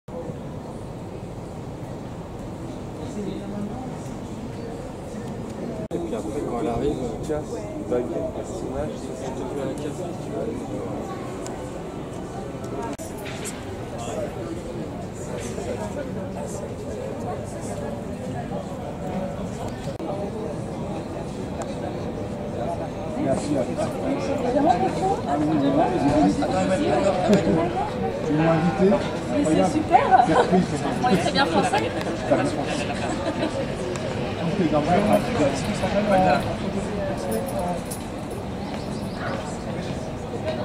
Chris Rankin / Percy Weasley Harry Potter @ Paris 10 june 2023 fans meeting La Boutique aux 2 balais
Chris Rankin / Percy Weasley - Harry Potter @ Paris 10 june 2023 signature La Boutique aux 2 balais